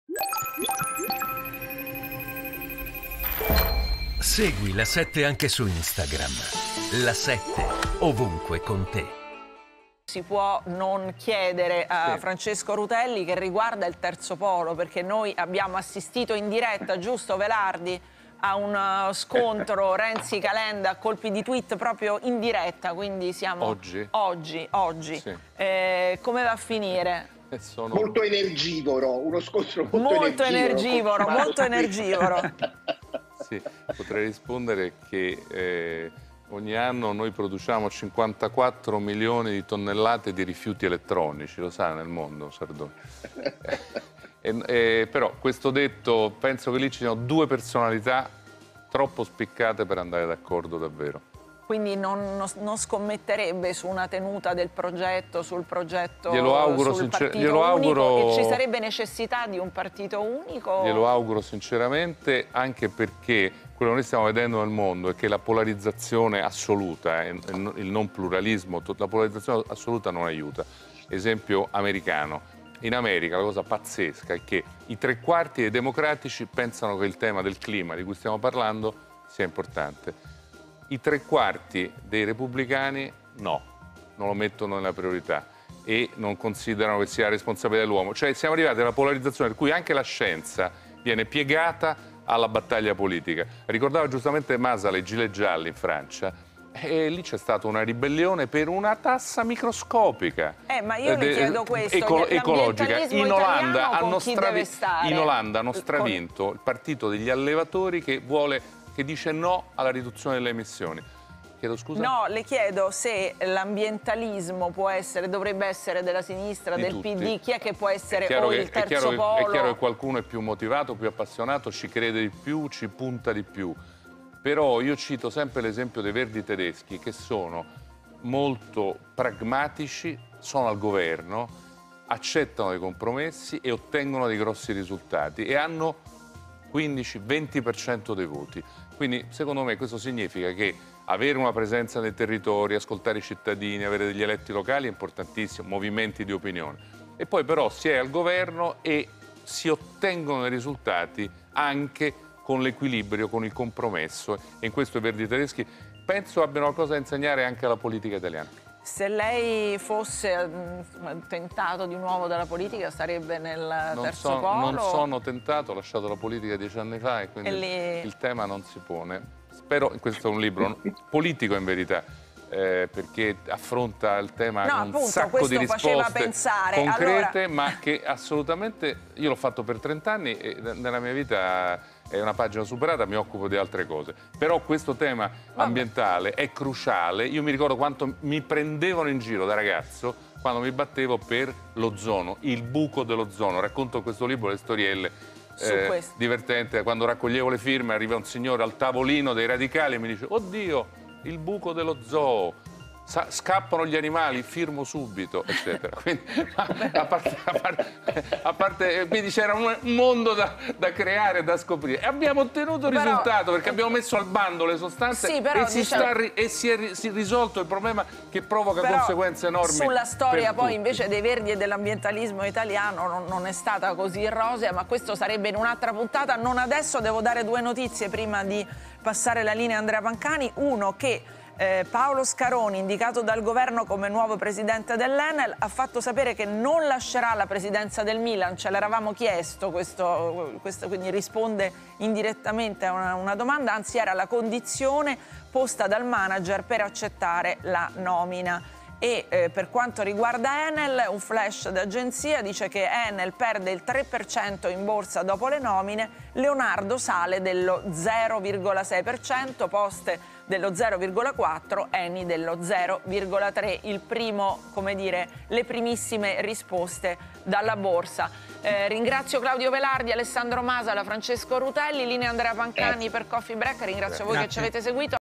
Terzo Polo, Rutelli: "Due personalità troppo spiccate per andare d'accordo"
Francesco Rutelli sul Terzo Polo e le continue lite tra Calenda e Renzi: "Due personalità troppo spiccate per andare d'accordo"